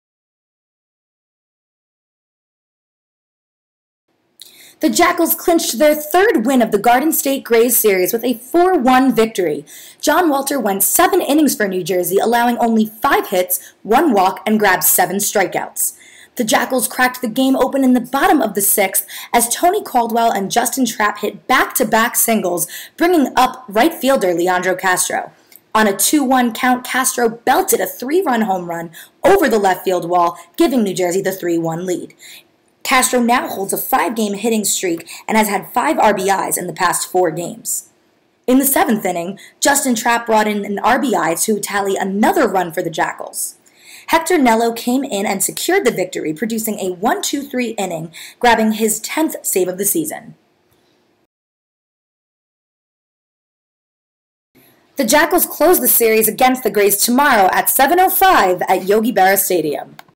NJ Jackals VS Garden State Grays 7/1/15
John Walter took the mound as the NJ Jackals took on the Garden State Grays in the fourth game of this five game series. Walter pitched 7 innings, gave up one run, and had 7 strikeouts. The Jackals offense came to life in the 6th inning as Leandro Castro belted a 3-run home run to give the Jackals a 3-1 lead. The jackals wouldn't look back and added another insurance run in the bottom of the 7th on a Justin Trapp RBI single. Hector Nelo would close the game in the top of the 9th with a 1-2-3 inning. The Jackals take on the Grays in the last game of the series tomorrow night at 7:05pm.